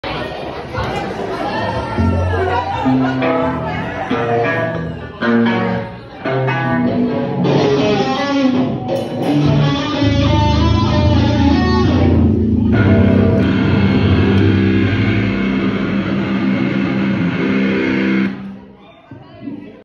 Dumb Youth live at Cosmic &Grounded 4-8-23 crowd ambience
Part one of Dumb Youth playing farewell/charity show “Mosh for what’s right” at Cosmic and Grounded on 4-8-23.